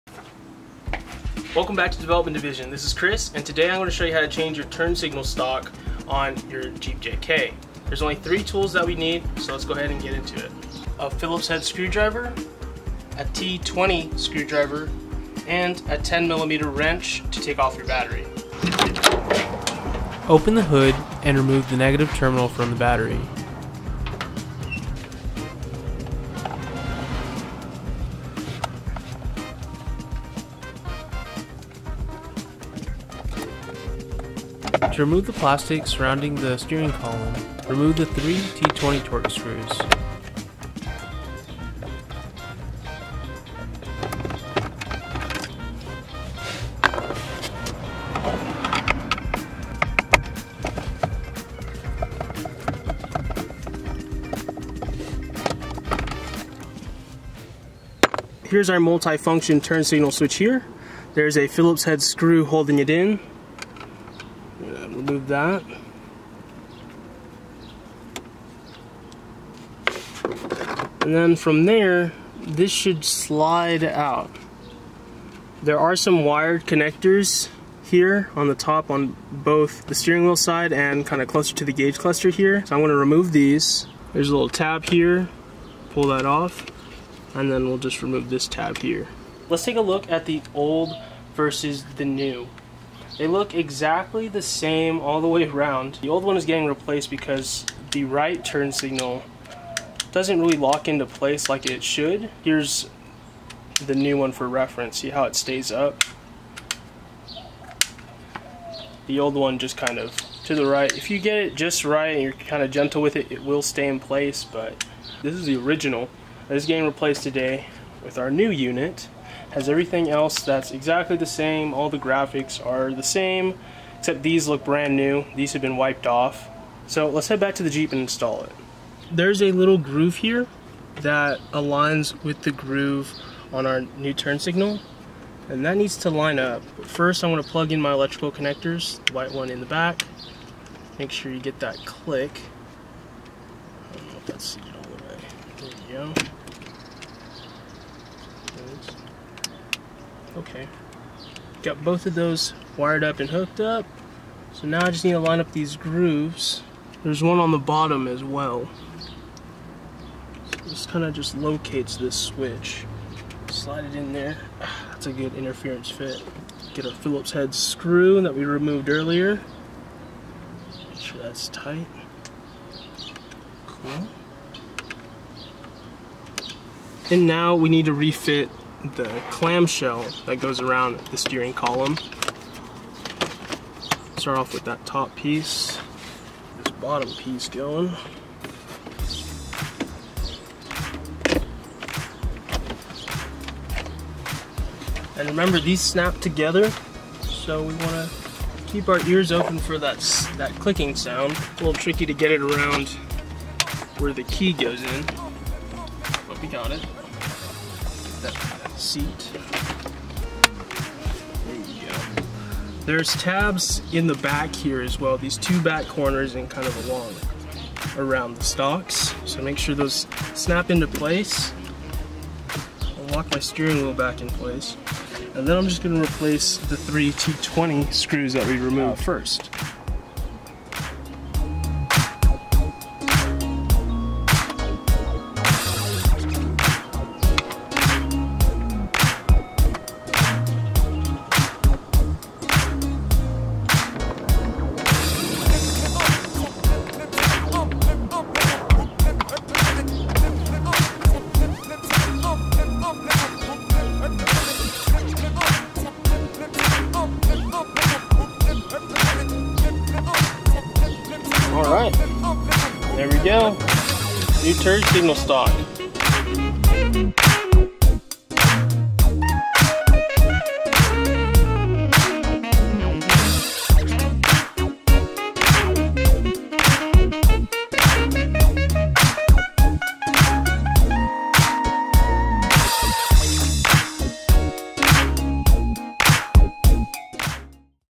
How To Replace Turn Signal Stock (Multi-function Switch) | 2007-2018 Jeep Wrangler JK
Welcome back to Development Division.

\\  Today I will show you how to replace a Turn Signal Switch in a Jeep JK Wrangler.

Part Number: 68015101AG
Link to replacement [OMIX]

Tools Needed:
#2 Phillips-head screwdriver
T20 Torx screwdriver
10mm wrench (to disconnect battery)